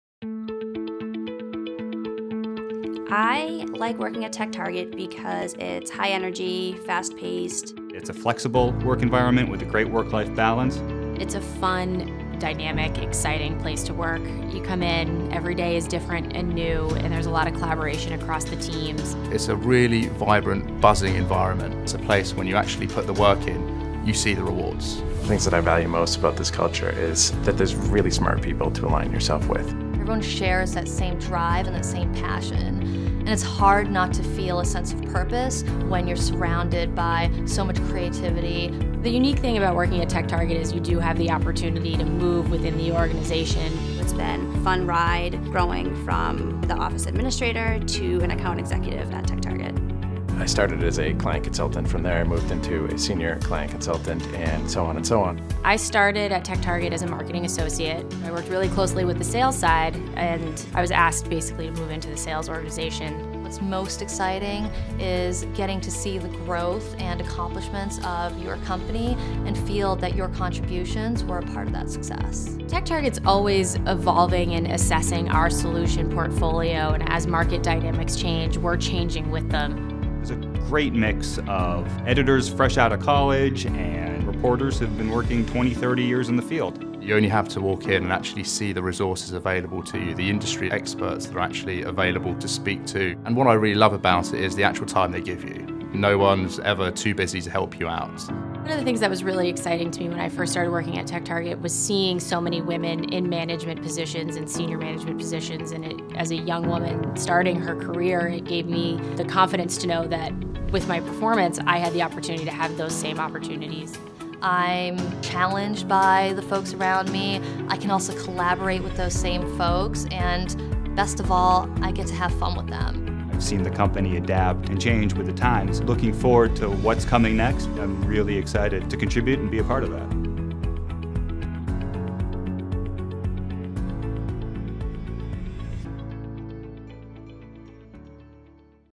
TechTarget Employees: In Their Own Words
What makes TechTarget such a dynamic place to work? Hear our employees describe the mix of people, their creativity, passion, and sense of purpose.